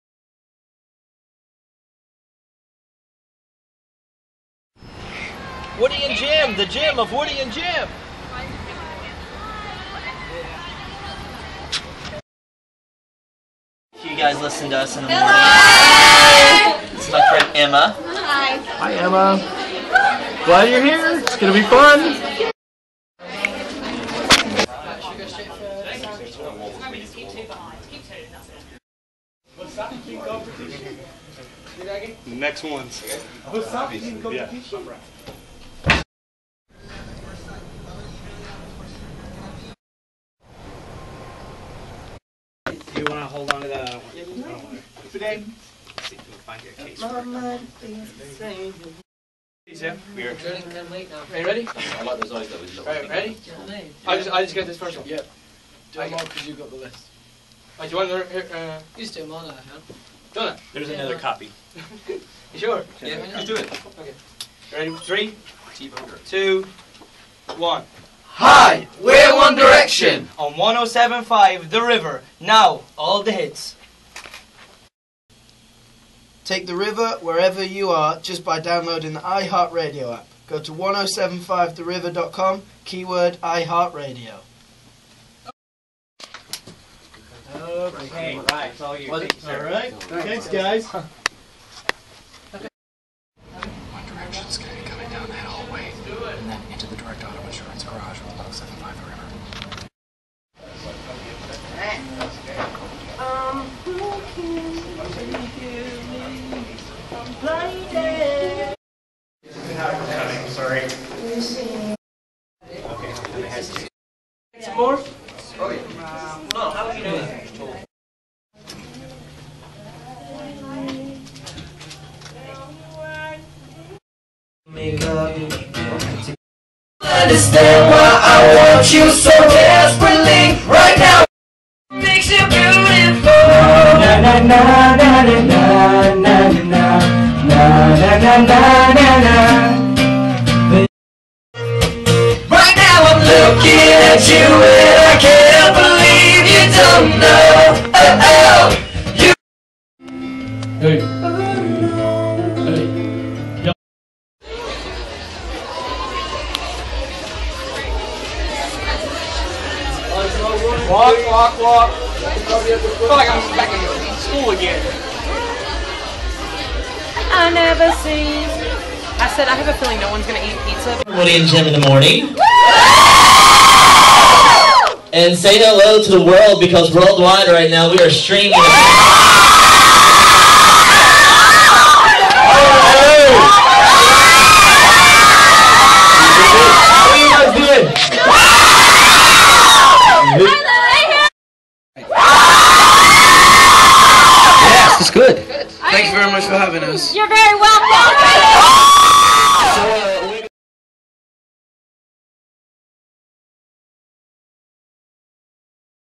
one direction behind the scenes at 107-5 The River, Nashville
One Direction spent the day with 107-5 The River in Nashville.  This is a behind the scenes access video.  Private moments.  Goofing around. Having fun.  And performing for River listeners in the Direct Auto Insurance Garage.